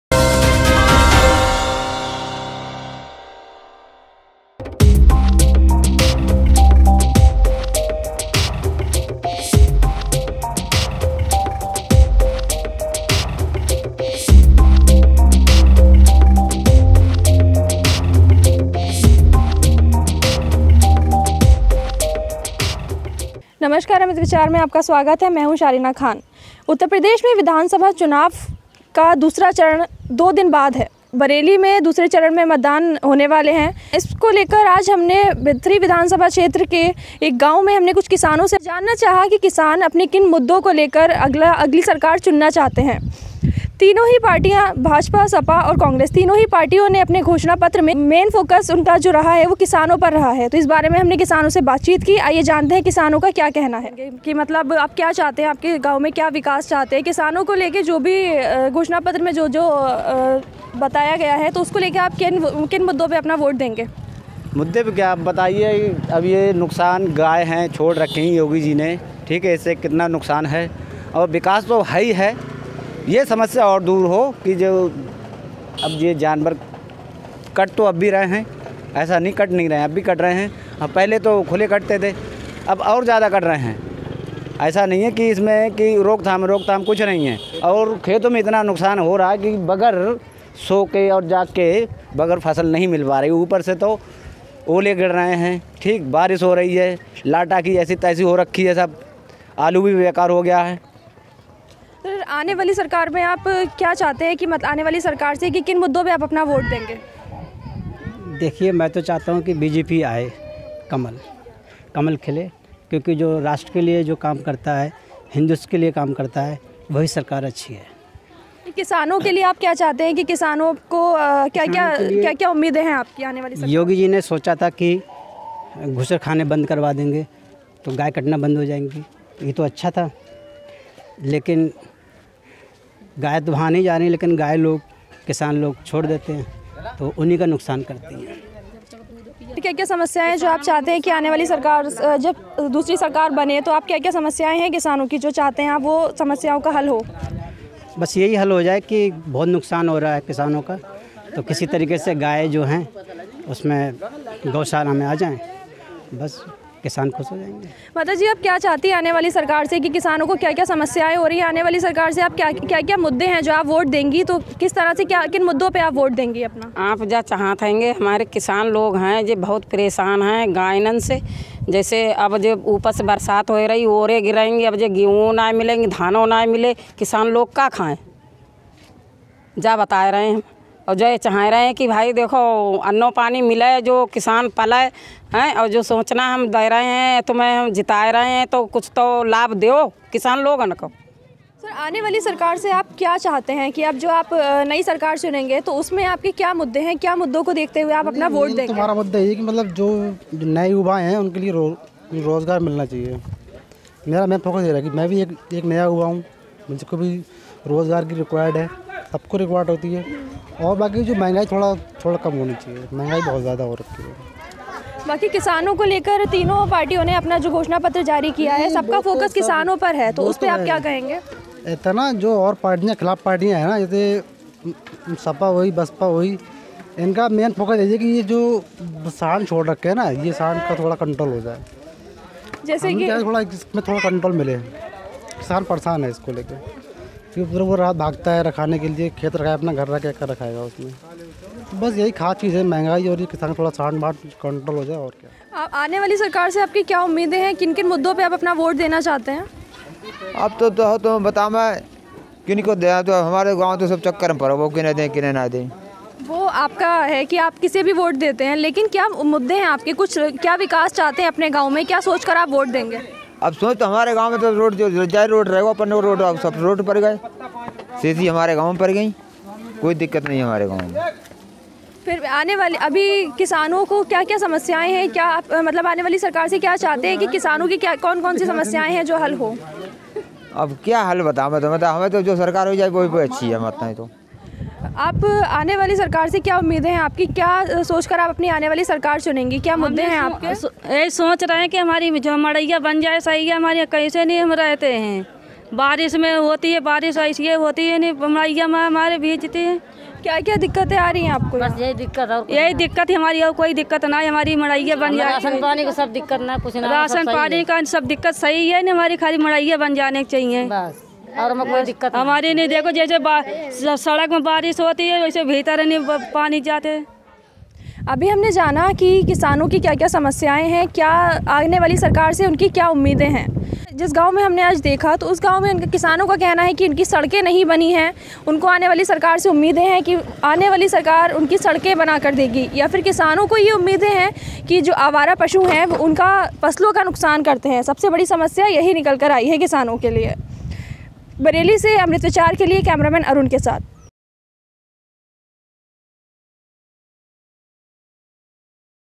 बरेली: जानिए...किसानों का सबसे बड़ा मुद्दा, क्या सोचकर करेंगे इस बार वोट? | Amritvichar
बरेली: जानिए...किसानों का सबसे बड़ा मुद्दा, क्या सोचकर करेंगे इस बार वोट?